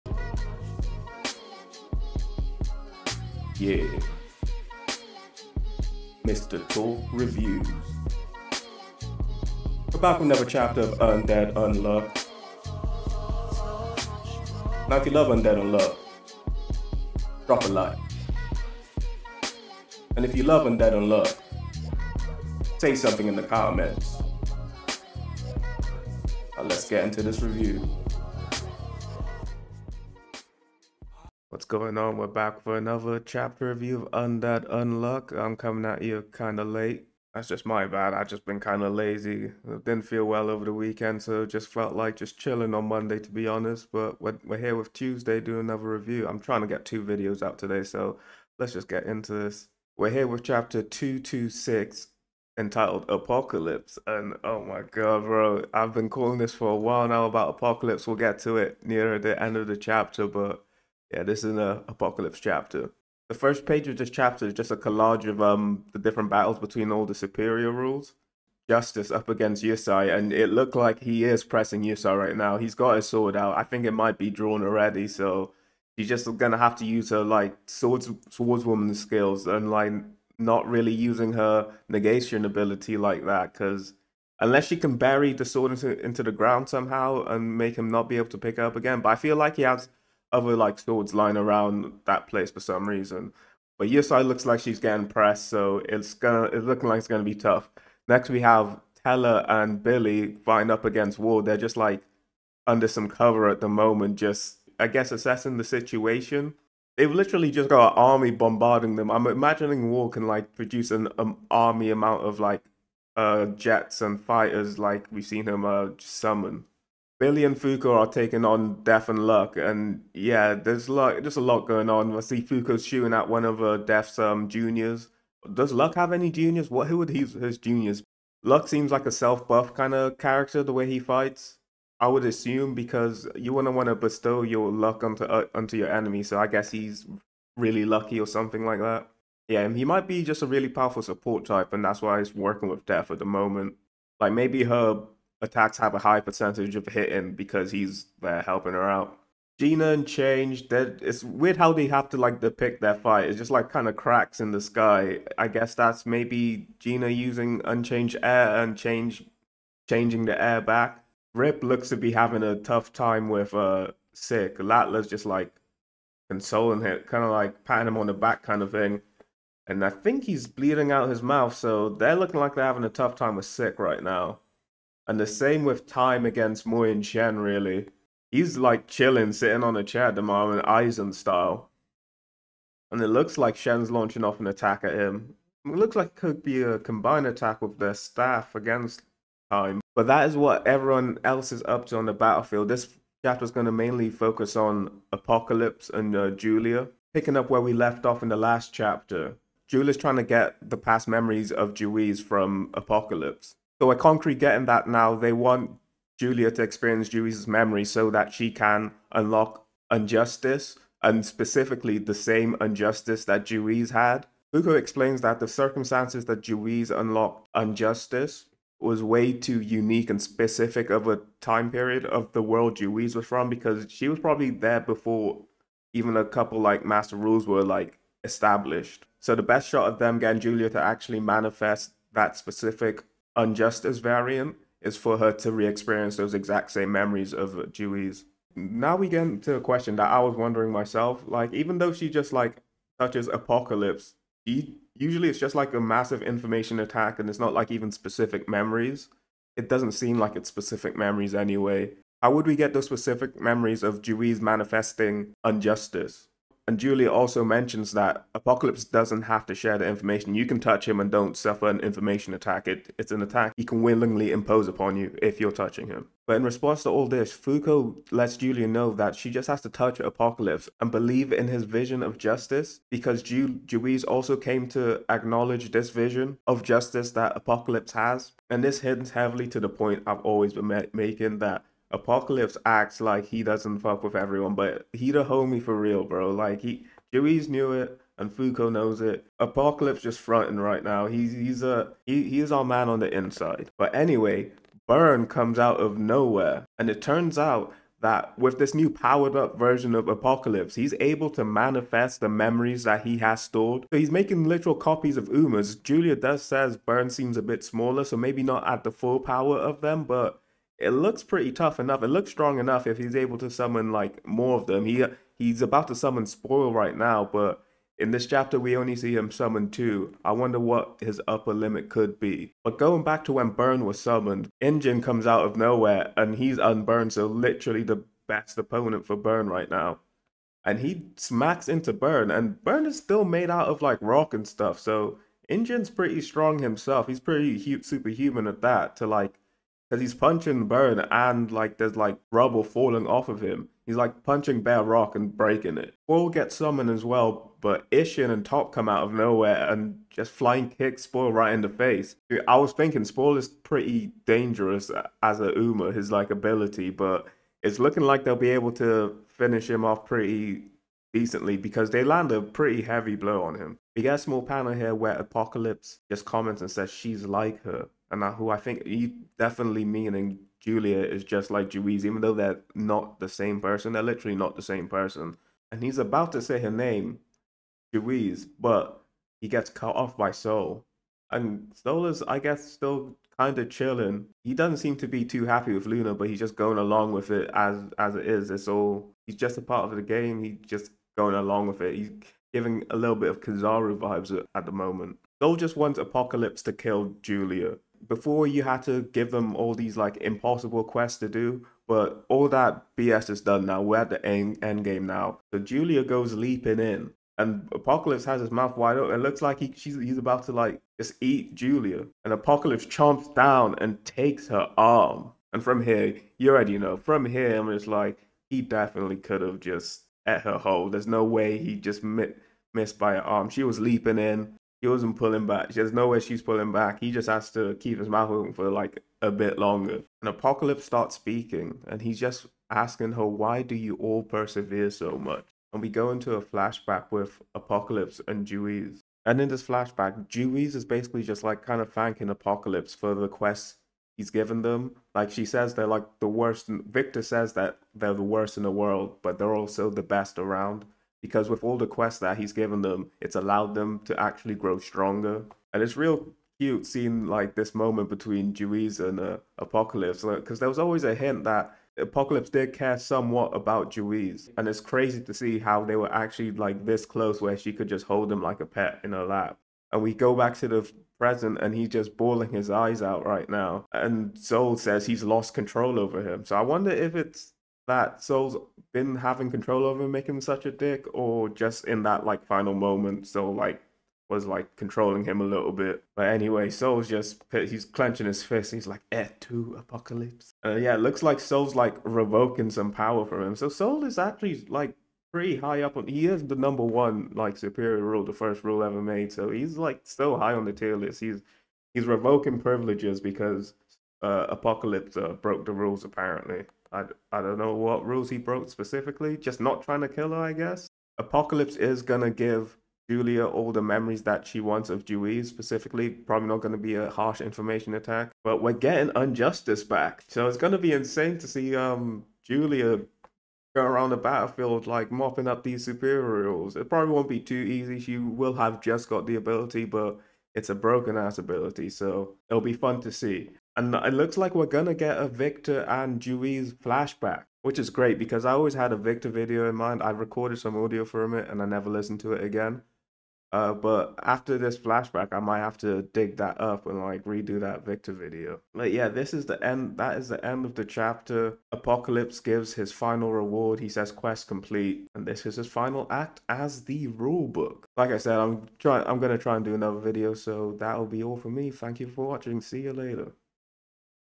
Apocalypse Shows His True Colours! - Undead Unluck Manga Chapter Review | The Rulebook!
Ragnarök continues and the Union are in the heat of battle. In order to manifest the unjustice ability Julia sets her sights on apocalypse. Will Julia be able to secure the memories she needs in order to manifest the correct version of unjustice?

00:00 Intro
00:51 Recap on match-ups
03:16 Julia Towards Apocalypse
04:54 Apocalypse shows some of his strength
06:57 Apocalypse true demeanour
08:10 End Of Apocalypse